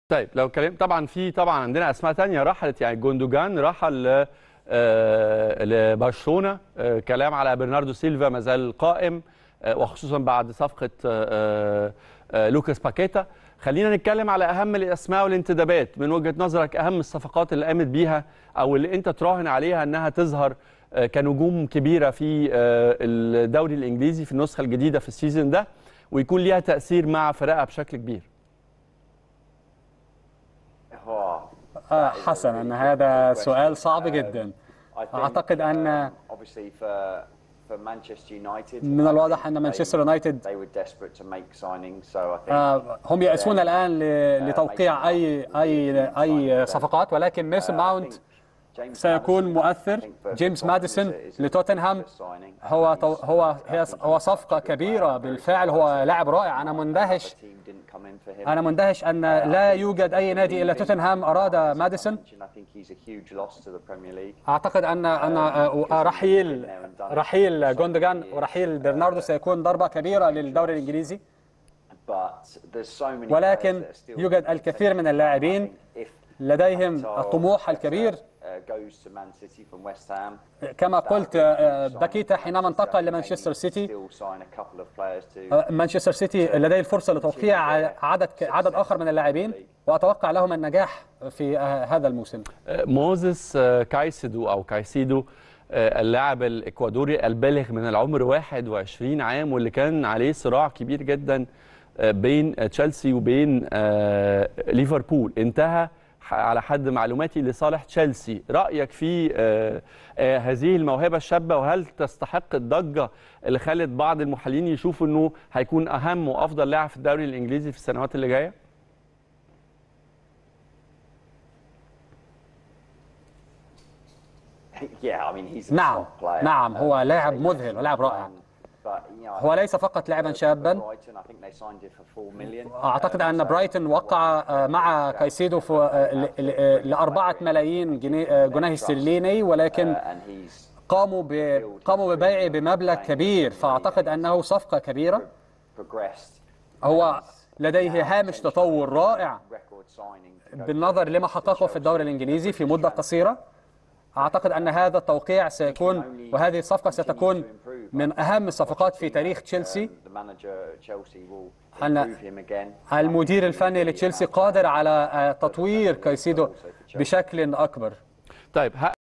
جرافيس نجم وست هام السابق: النجوم الراحلين عن الدوري الأنجليزي صوب البطولة السعودية كان لسبب مادي
مات جرافيس نجم وست هام وولفرهامبتون الأنجليزي السابق: النجوم الراحلين عن الدوري الأنجليزي صوب البطولة السعودية كان لسبب مادي | السادسة